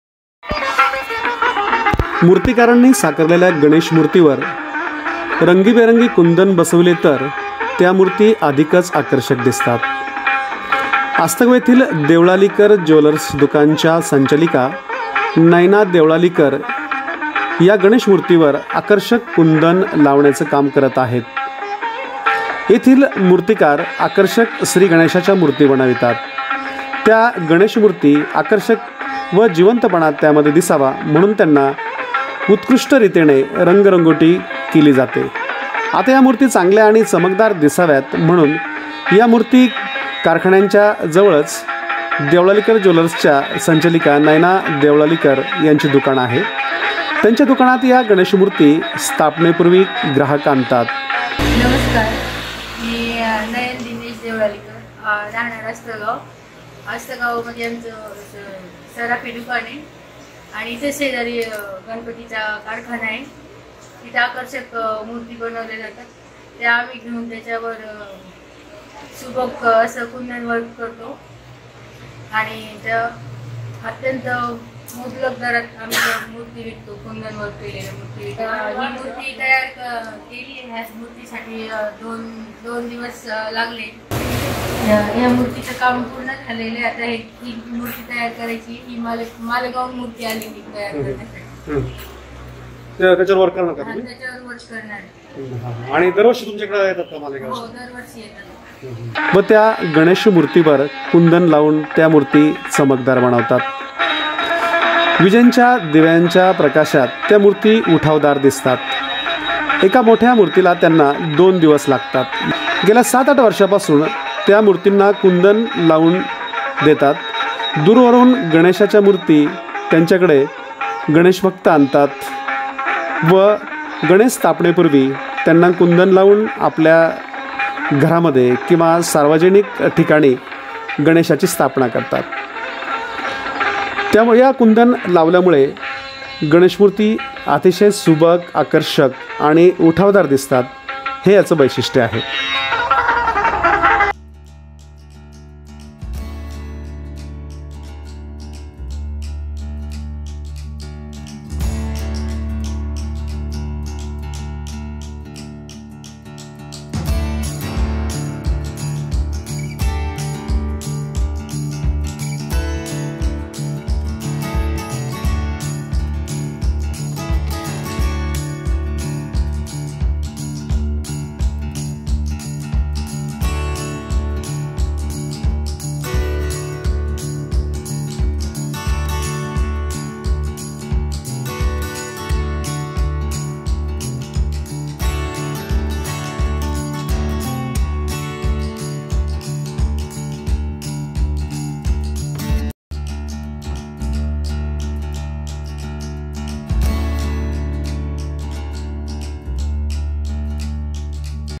अशी सजते गणेशमूर्ती | astagaon | ganpati story | mahendra jejurkar
गणेशमूर्तींवर रंगीबेरंगी कूंदन बसविले तर त्या अधिकच आकर्षक दिसतात. अस्तगाव येथील ज्वेलर्स दुकानच्या संचालिका नयना देवळालीकर या गणेशमूर्तीवर आकर्षक कुंदन लावण्याचा काम करत आहेत.
मूर्ती चमकदार दिसाव्यात म्हणून कुंदन लावले जाते. दिव्याच्या प्रकाशात त्यामुळे मूर्ती उठावदार दिसतात.